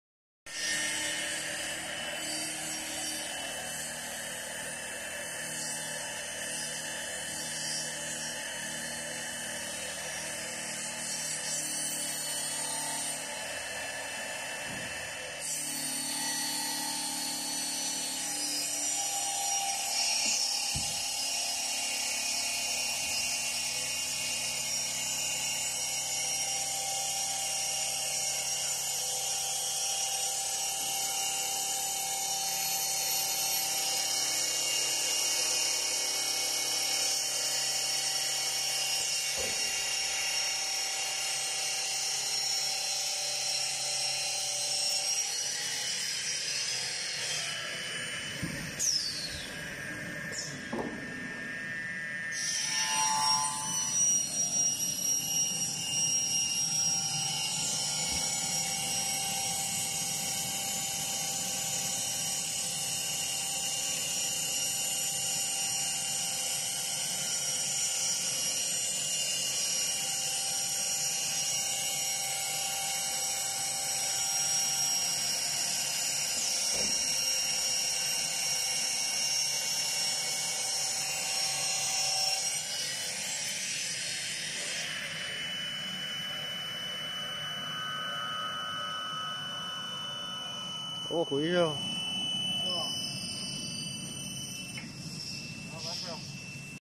Customized suntec wood cnc router for hard wood cutting with saw
customized saw 120mm
working 1300*2500mm
vacuum table  with press wheel
NCcontroller